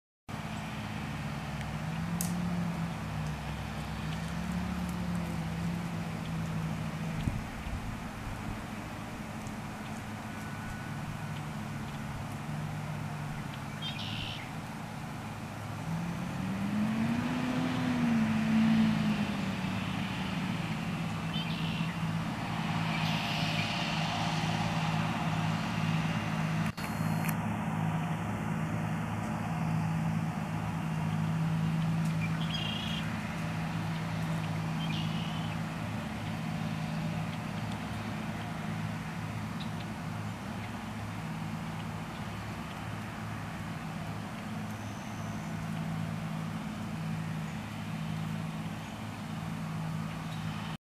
John Deere tractors ripping
John Deere 8285r and 8225r doing fall tillage with 4 row 38" ripper bedders.